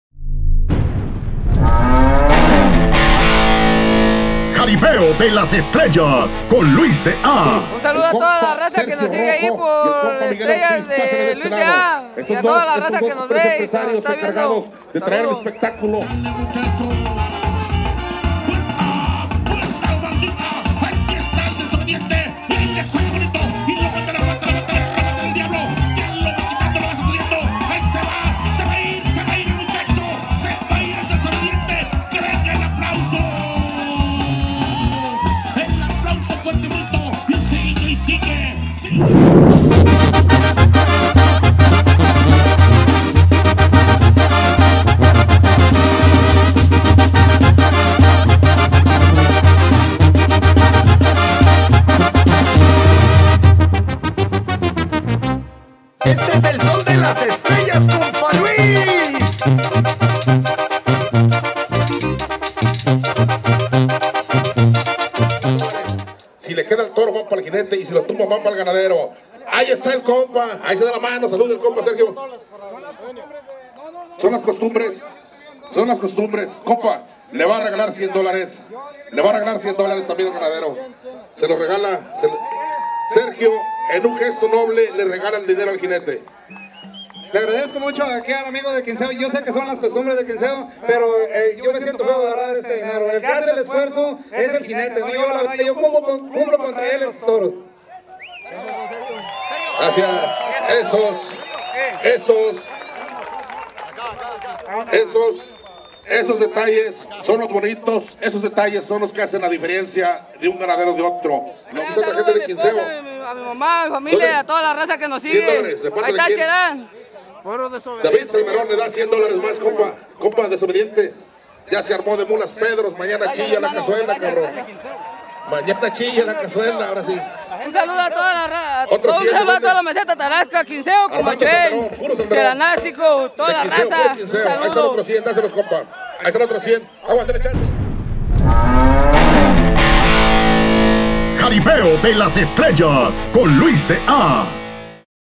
DESOBEDIENTE DE CHERAN VS AGUAJE ALTERADO
DESOBEDIENTE DE CHERAN ANDA INPARABLE  QUE BUENA MONTA SE AVENTO Y LA GENTE LO PREMIO CON  DINERO  MONTO AL AGUAJE ALTERADO